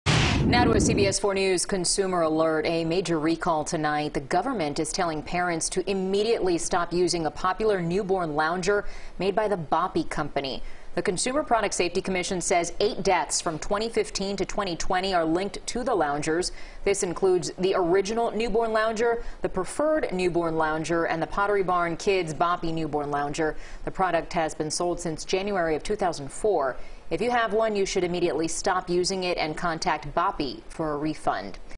Boppy Loungers Recalled
The Boppy loungers are being recalled after being linked to 8 infant deaths since 2015.